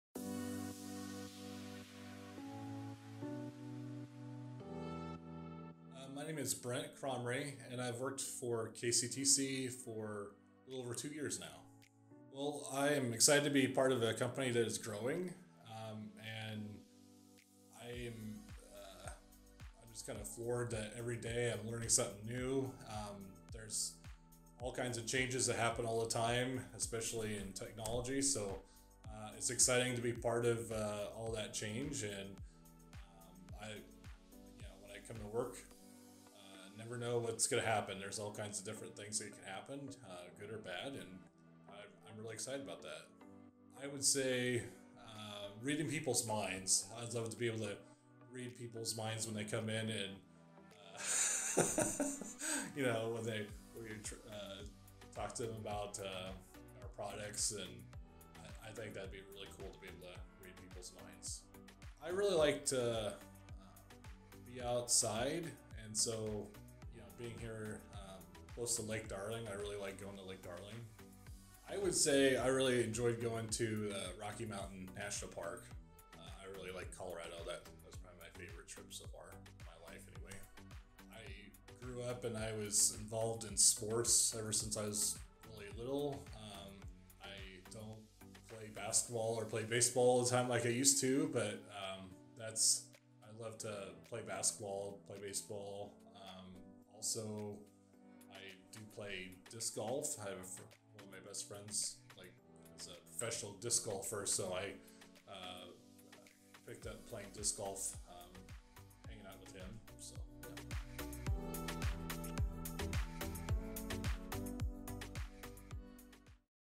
Brent Kromrie, KCTC Employee
KCTC staff member, Brent Kromrie, shares a little about himself and what he enjoys about working at Kalona Cooperative Technology Company.